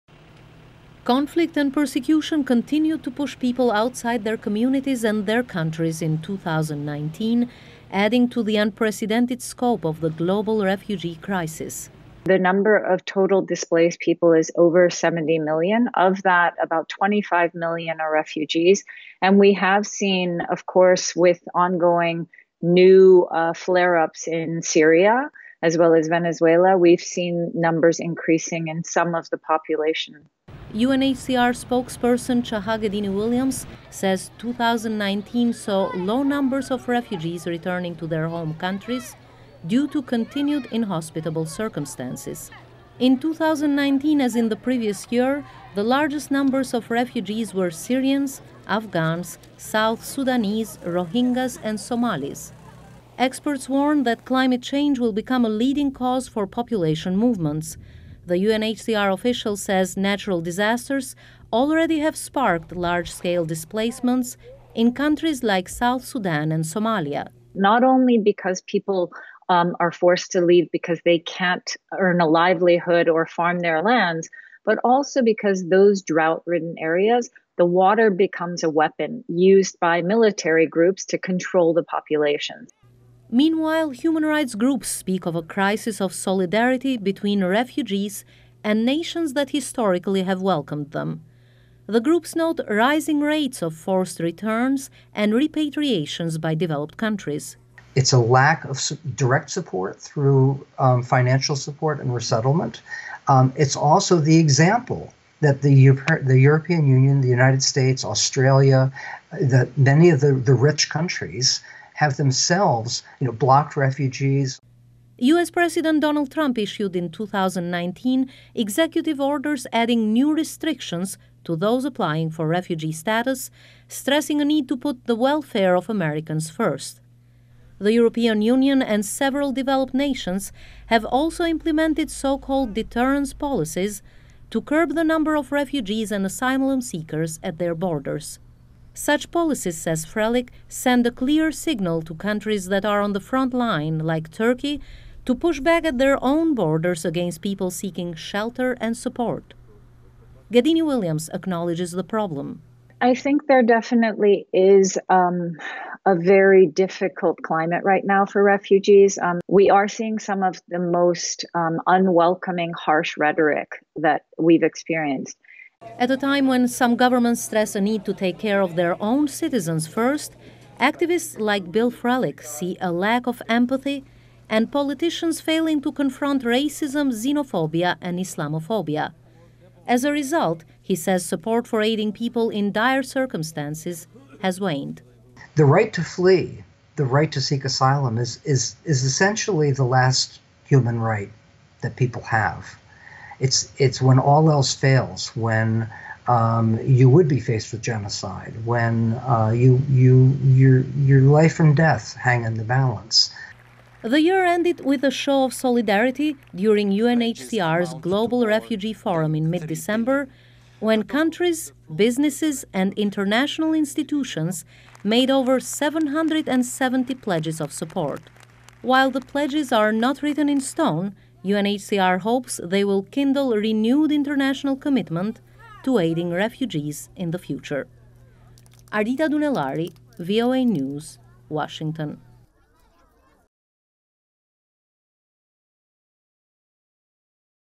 Ranks of Refugees Grow in 2019 Amid 'Crisis of Solidarity'
The worldwide refugee crisis continues to deepen. According to the United Nations, the world is witnessing "the highest levels of displacement on record." Spurred by conflict and persecution, the numbers of displaced are expected to rise further in the future, as climate change pushes people out of their homes.  VOA's Ardita Dunellari reports.